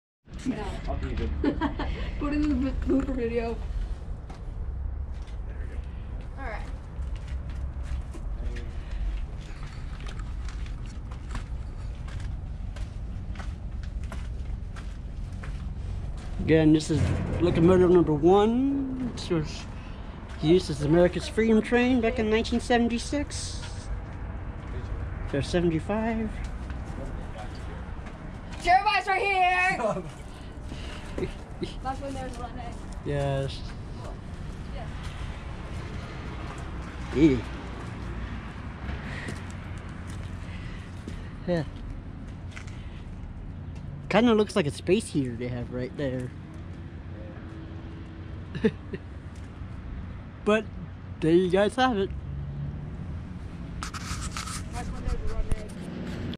THE AMERICAN FREEDOM TRAIN LOCOMOTIVE 2101 STEAMLOCOMOTIVE AMERICANFREEDOMTRAIN #2101
on this episode of steam enthusiast productions we will... see the American freedom train locomotive #1 the t-1  reading 2101